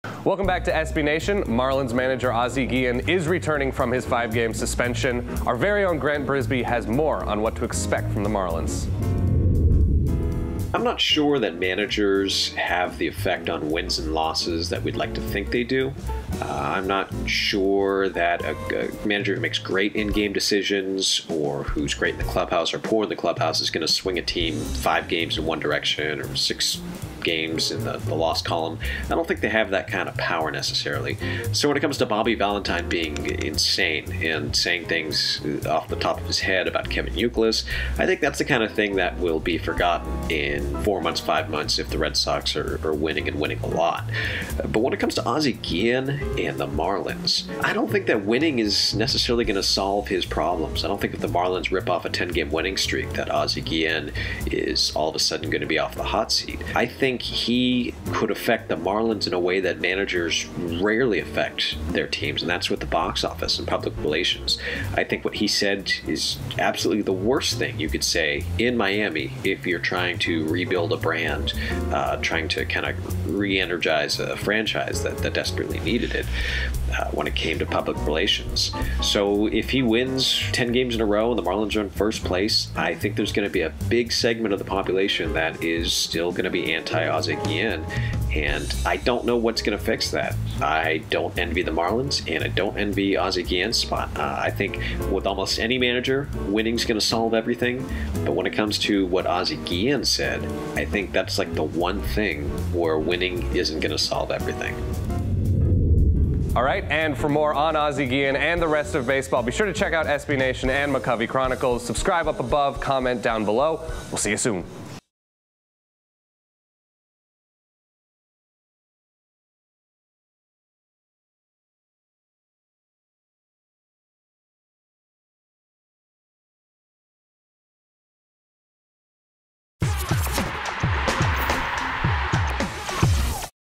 Ozzie Guillen Returns From Suspension
Grant Brisbee weighs in on Ozzie Guillen's return to the Marlins after a 5 game suspension.

Steve Mitchell-US PRESSWIRE Derick E. Hingle-US PRESSWIRE Kim Klement-US PRESSWIRE Bob DeChiara-US PRESSWIRE